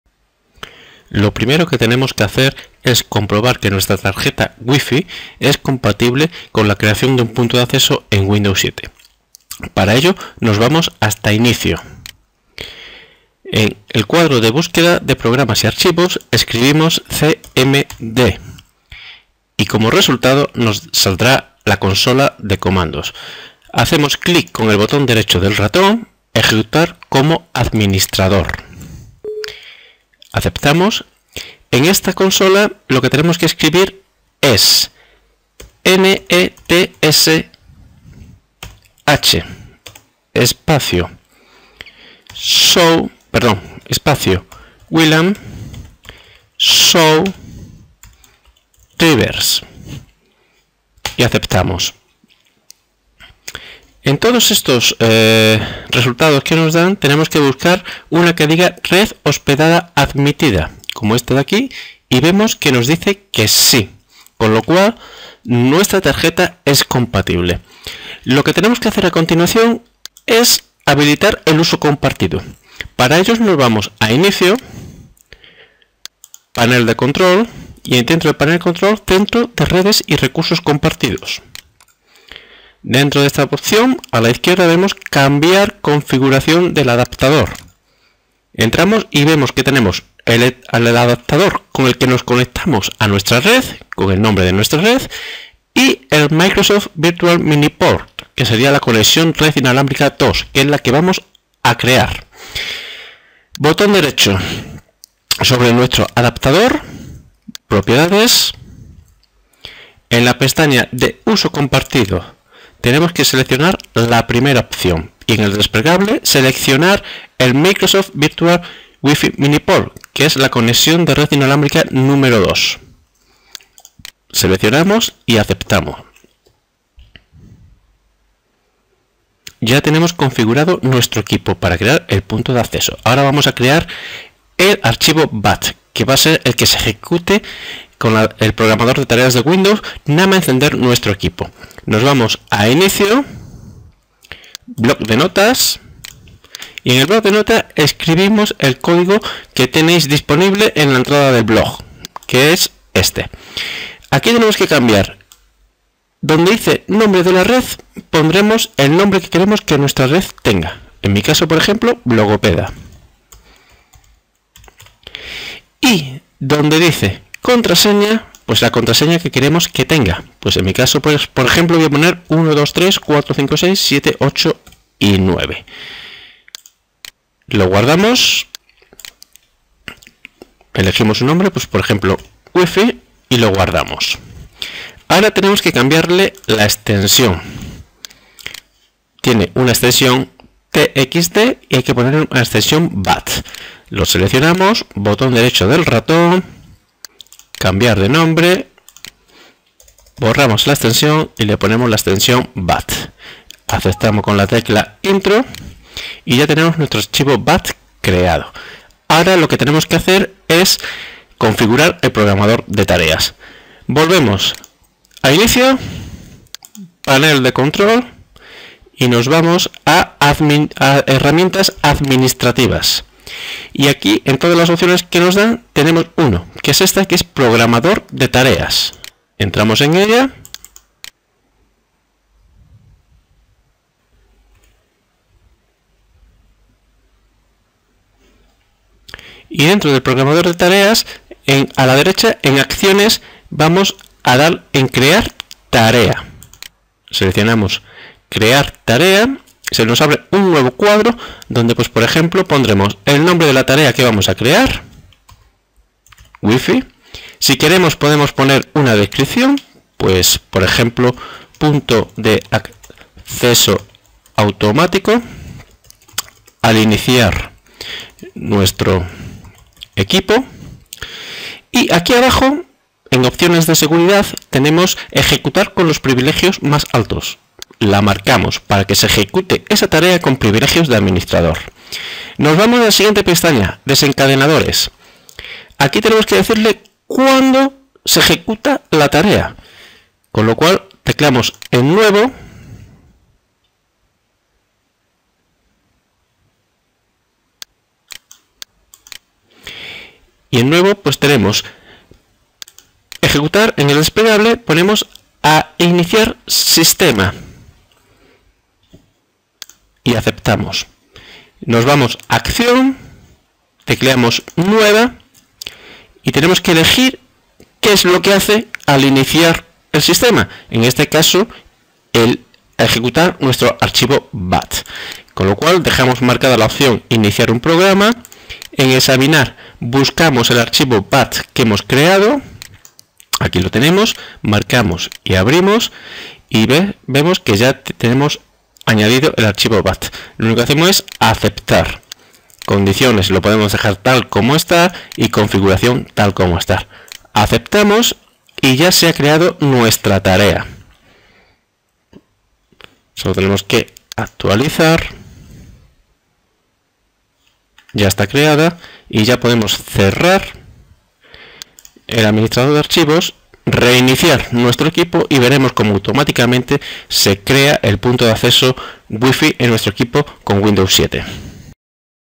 Crear punto de acceso wifi en windows 7 de forma automática al iniciar el equipo
Cómo crear un punto de acceso wifi en un equipo con Windows 7 de forma automática al iniciar el equipo utilizando el programador de tareas y un archivo .bat